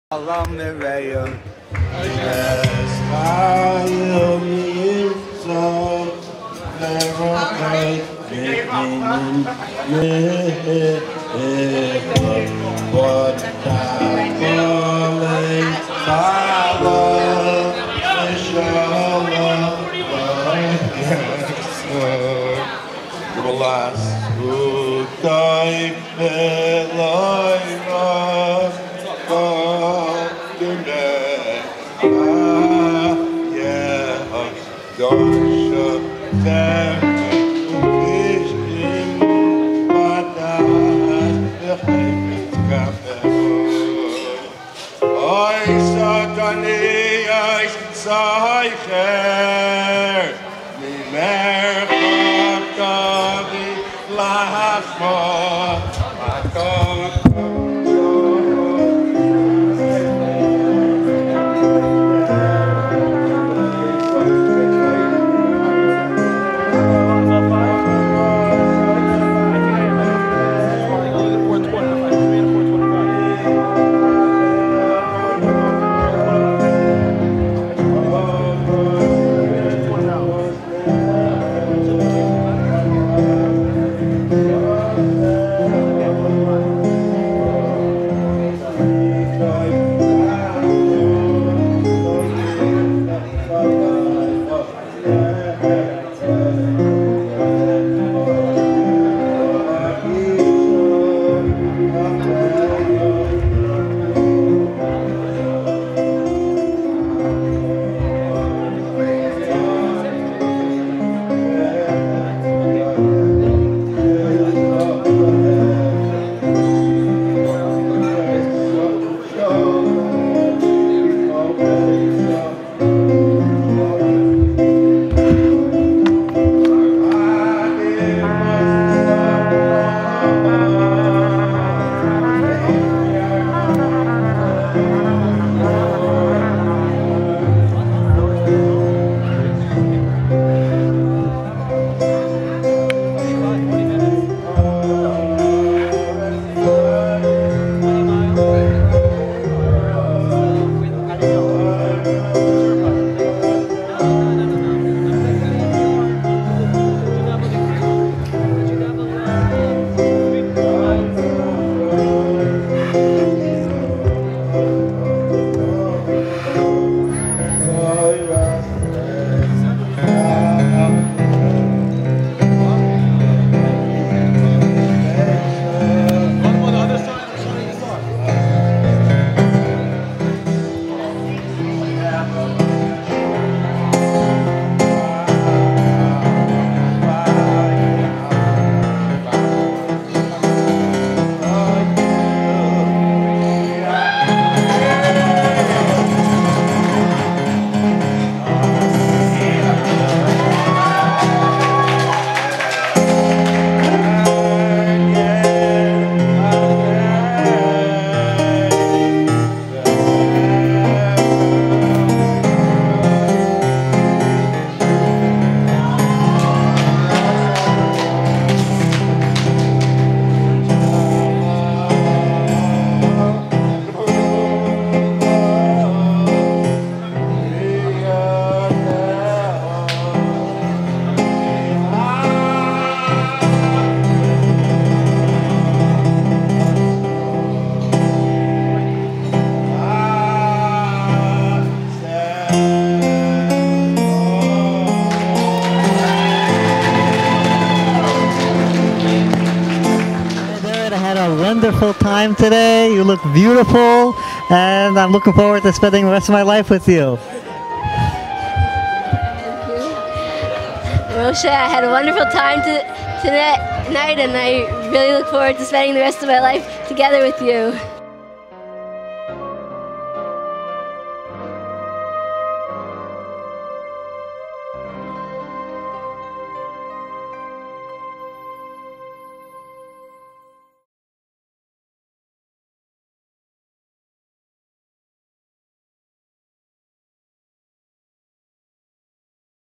Aderet & Moshe Wedding Highlights
Brides Father singing to his wife.